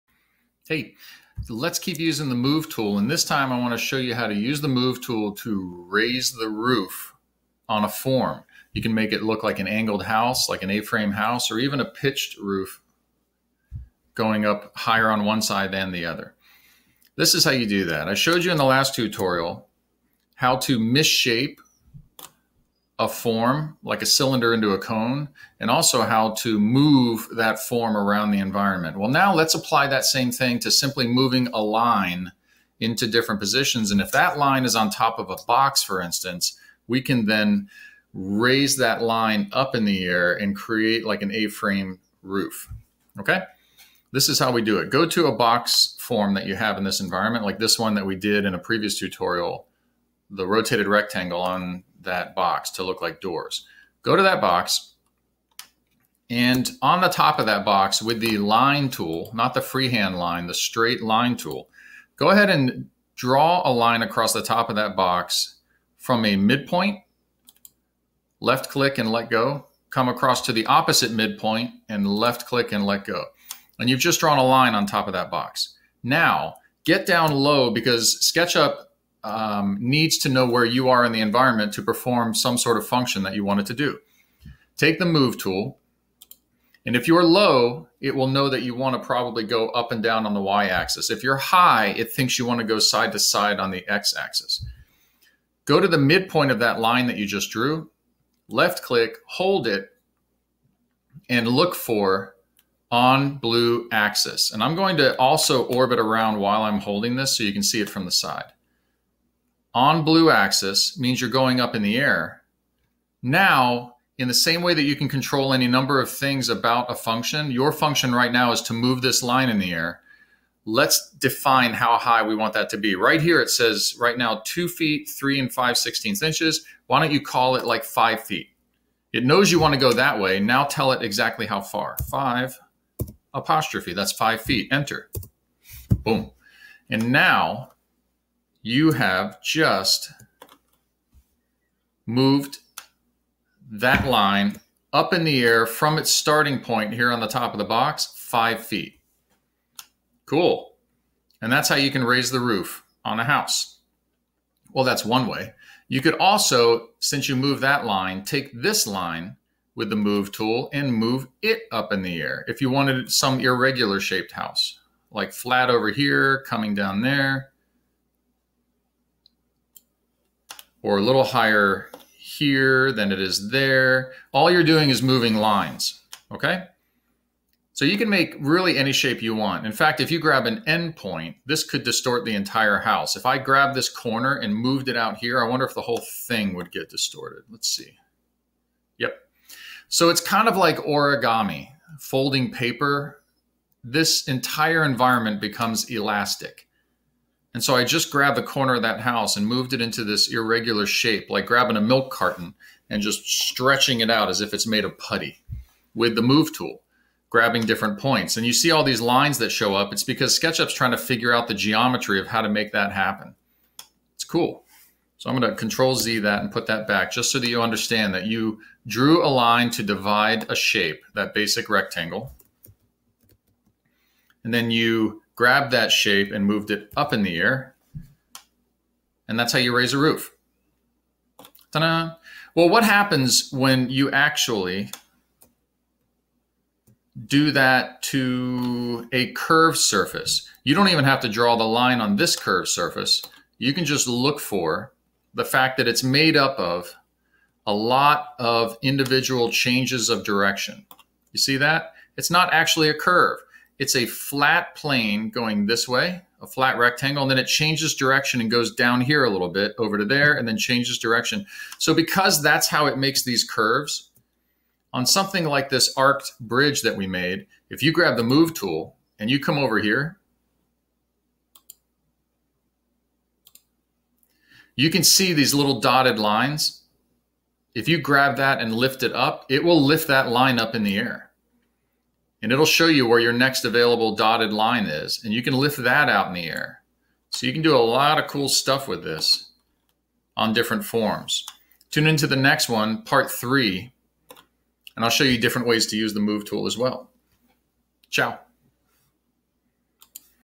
Lesson 10.1: Move Tool Part 2 - Raise the Roof!
Learn to create a raised roof with the Move Tool
Note: SketchUp changed their toolbars since this video was made.  If you don't see the toolbar in the same layout as this video, look for the tool in the three dots icon ( ... ) near the bottom of the left-hand toolbar.  A pop-out window will appear with the additional tools.